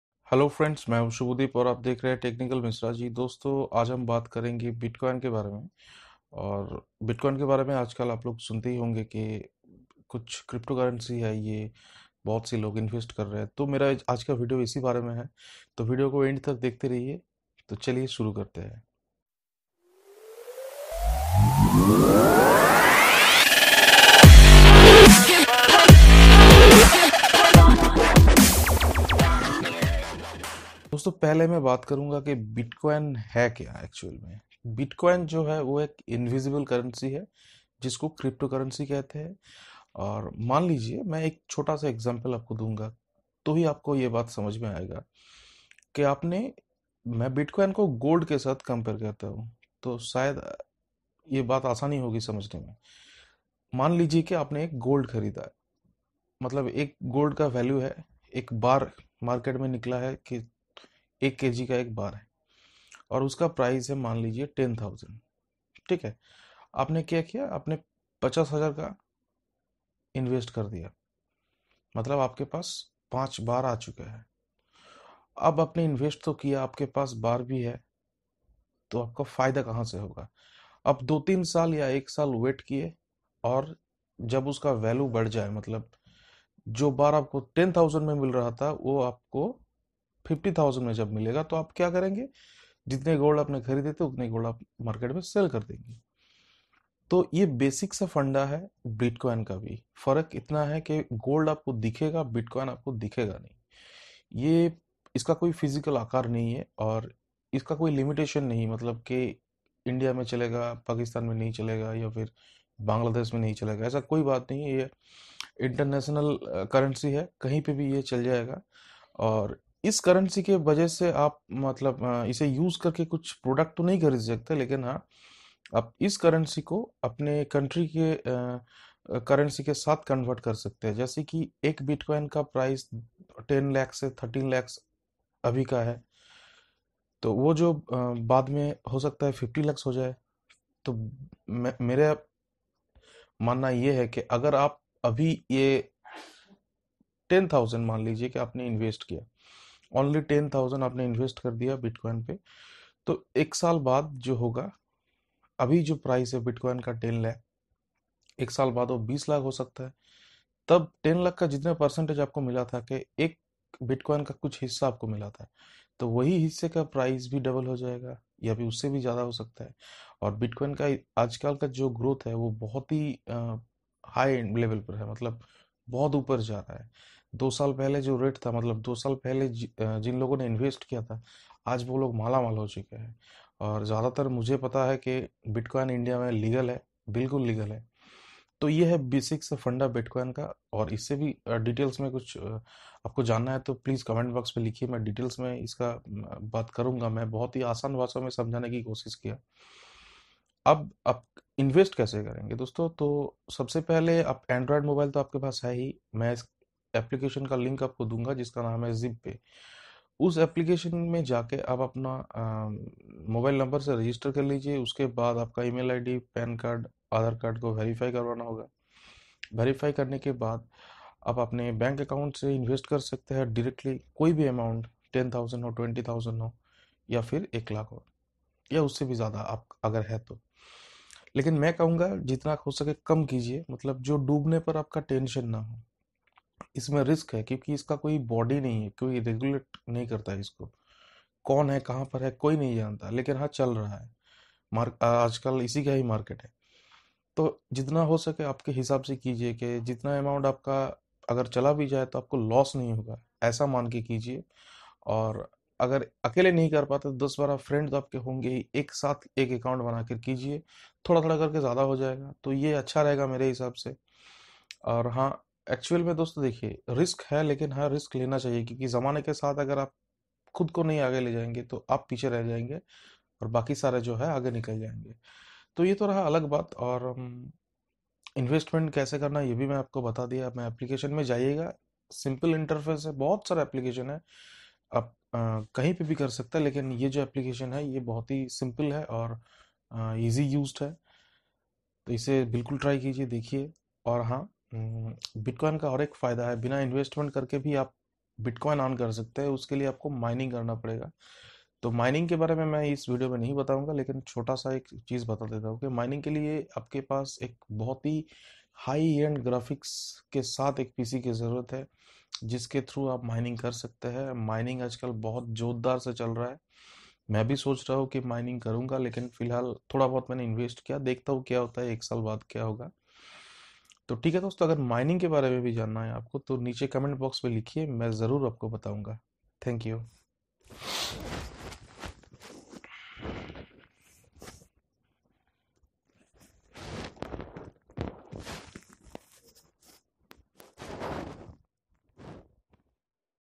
What is BITCOIN and how to invest on it [HINDI]
Learn everything about Bitcoin in hindi